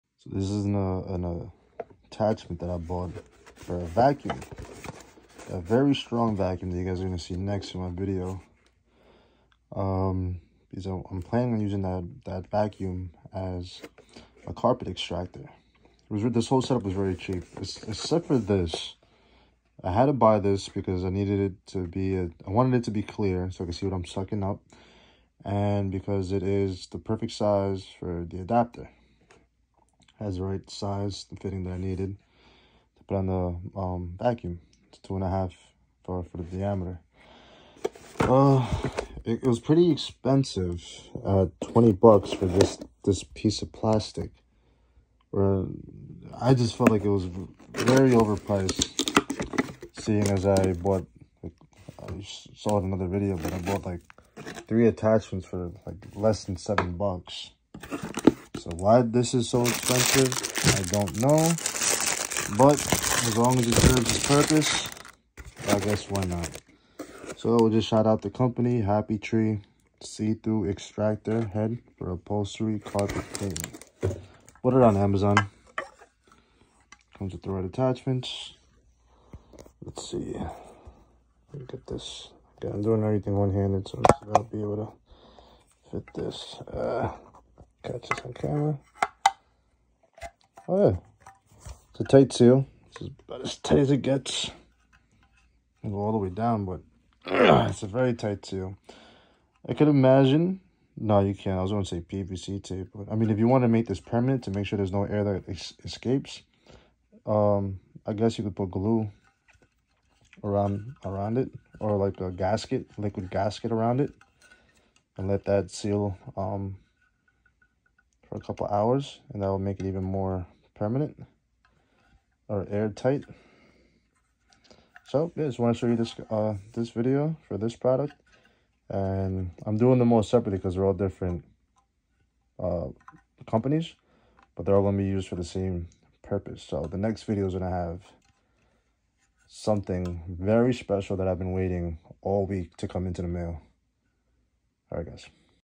Carpet extractor accessory for car detailing
All though I think this piece is a little bit pricey at 19.79 for a small piece of plastic, it’s still a necessary piece when needed a plastic see through carpet extractor vacuum head.